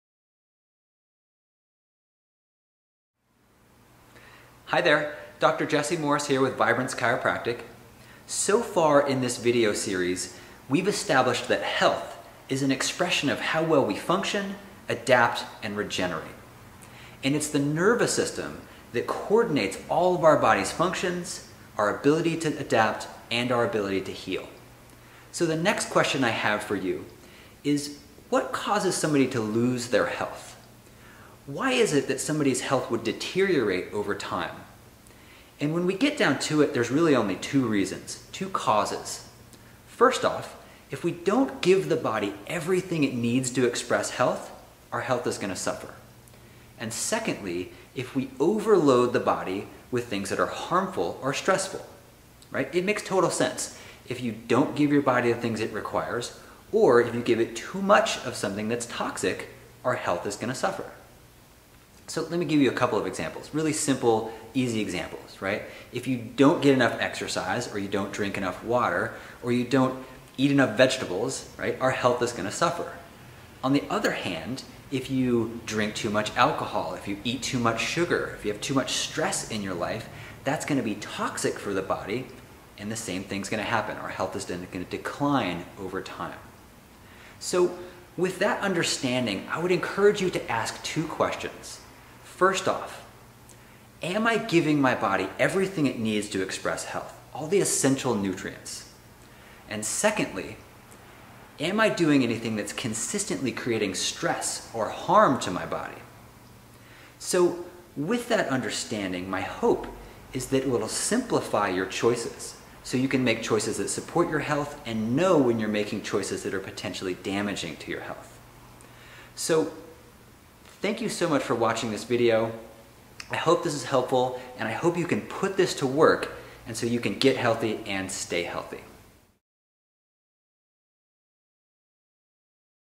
2 Ways That We Lose Our Health
There are essentially only 2 ways that we lose our health. Too much of something that is toxic for the body or not enough of what what the body requires.

Dr. Jesse Morris is a chiropractor at Vibrance Chiropractic in San Anselmo California.